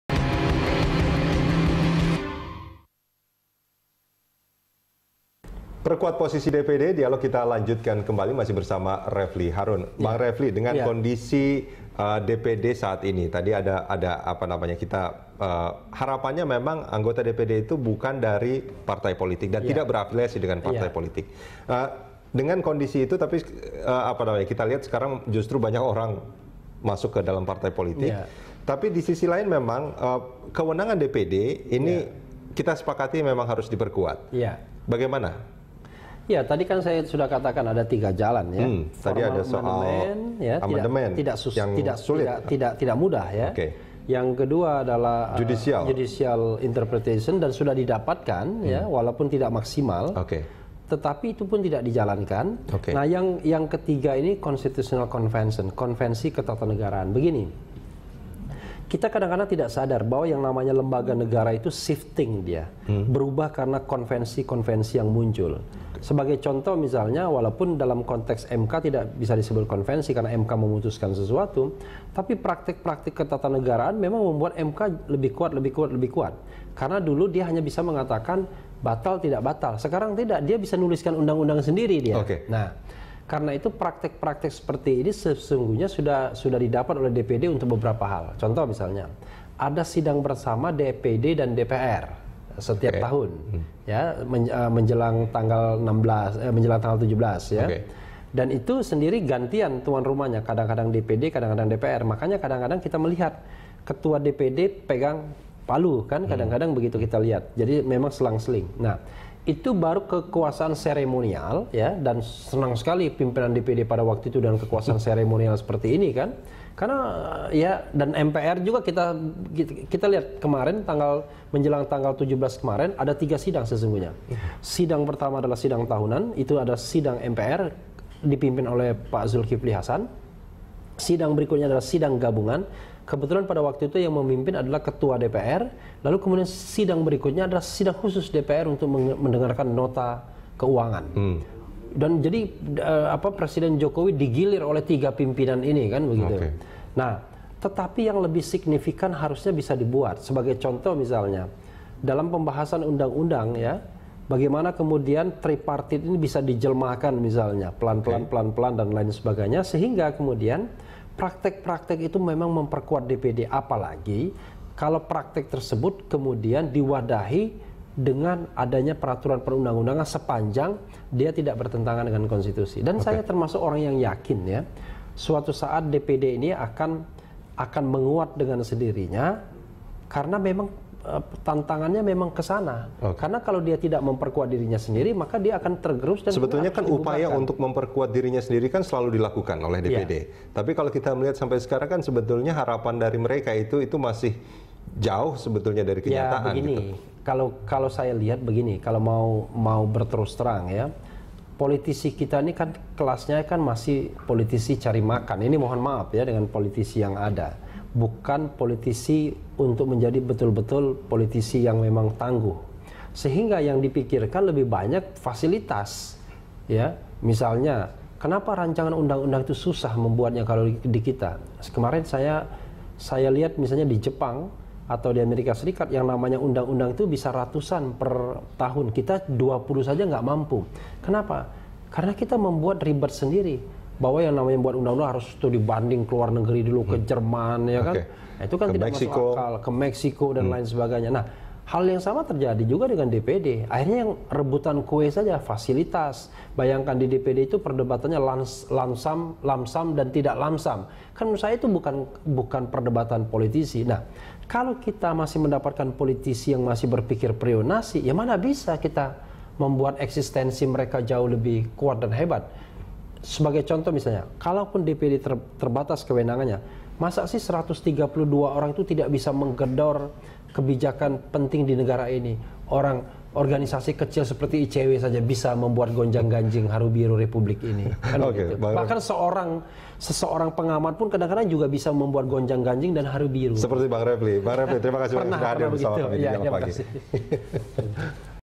Dialog: Perkuat Posisi DPD #3
Keberadaan Lembaga Dewan Perwakilan Daerah adalah hal penting dan strategis karena tertuang dalam rumusan konstitusional. Selain itu, DPD juga menjadi wakil pembawa isu kedaerahan untuk dibawa ke Jakarta agar menjadi pertimbangan dan ikut menentukan arah pembangunan Nasional. Namun dalam praktiknya, geliat DPD kalah pamor dan kalah greget dari lembaga Dewan Perwakilan Rakyat.

Facebook.com/BeritaSatuTV
Youtube.com/BeritaSatu